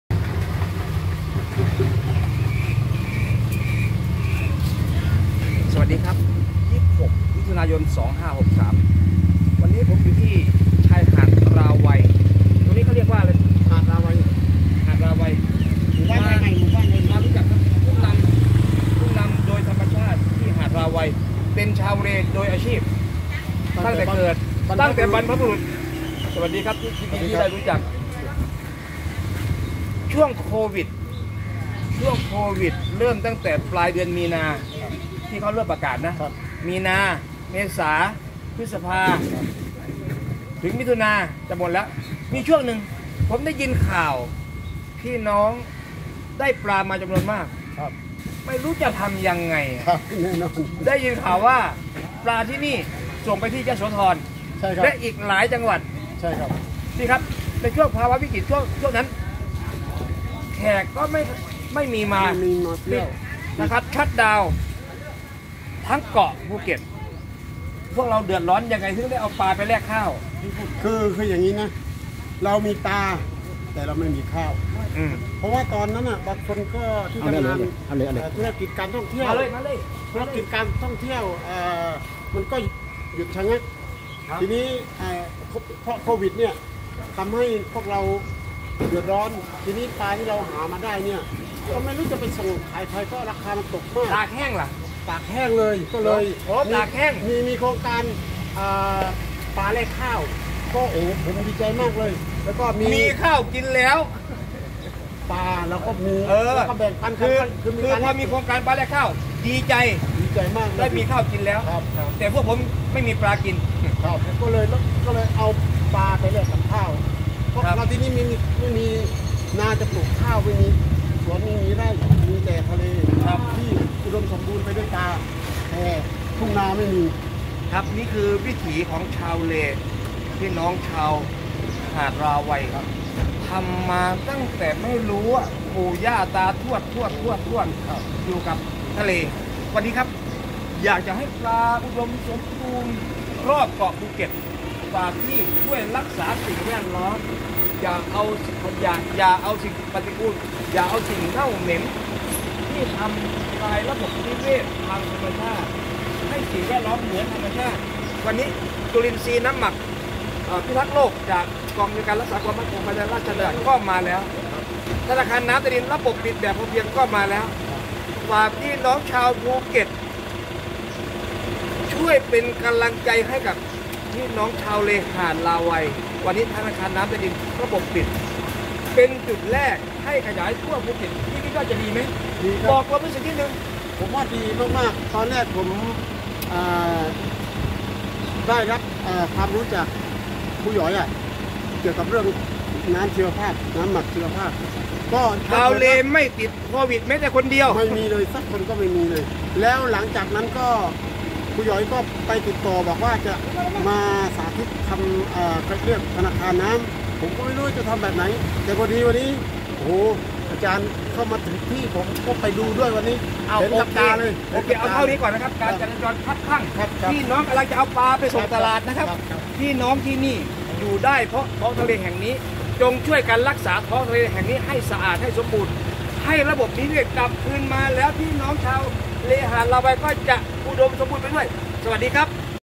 วิถีชาวเล หาดราไวย์
กอ. รมน. ราชอาณาจักร์ ชี้ทางออกให้กับพี่น้องชาวเล ทึ่หาดราไวย์ ฟื้นสิ่งแวดล้อมให้ระบบนิเวศกับคืนมา